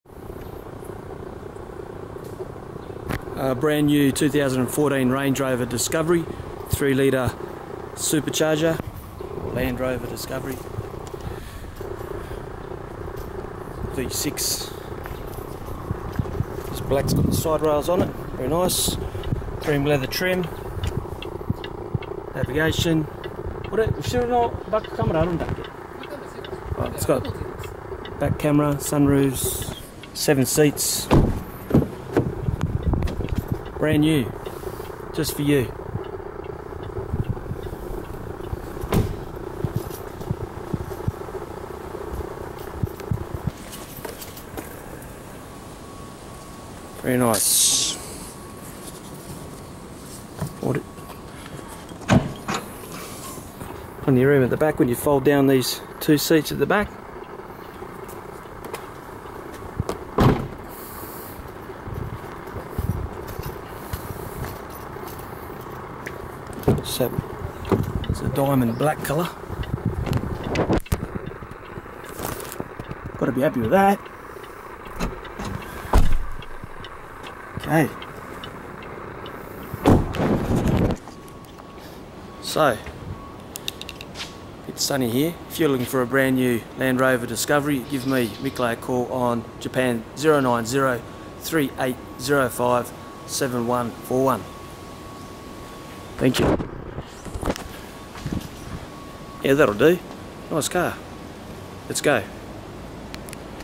Buy New 2014 Discovery 3ltr Super Charger - Tokyo Japan
Looking to buy a new Land Rover Discovery in Tokyo Japan?  Call me Mick Lay for a quote - depending on the timing stock is available for immediate delivery.